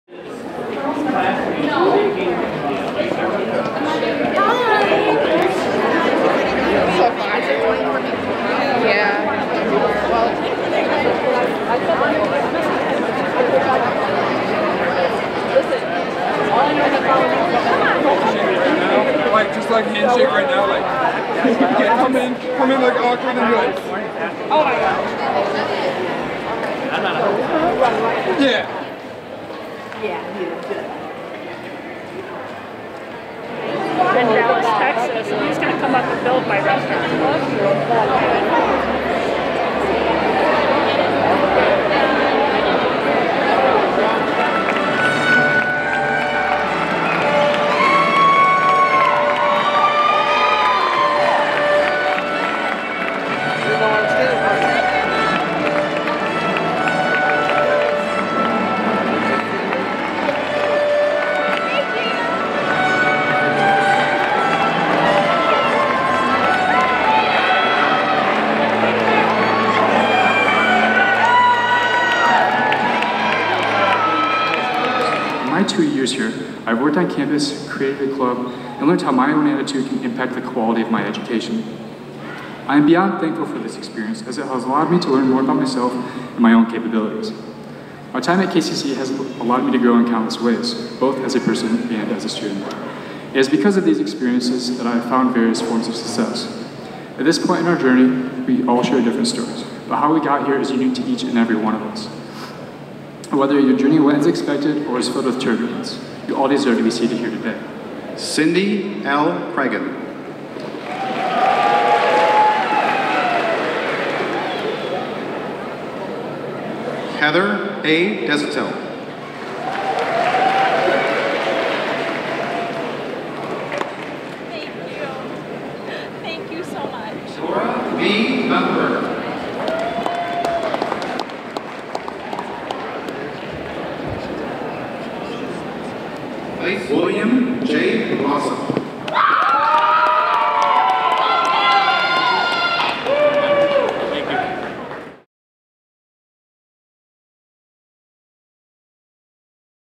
Kellogg Community College Commencement, May 9, 2019
Kellogg Community College celebrated the awarding of more than 1,000 degrees and certificates at the College's 2019 Commencement Ceremony May 9 at Kellogg Arena in downtown Battle Creek. More than 350 graduates participated in the ceremony. Congratulations graduates!

Video by Andy Fitzpatrick.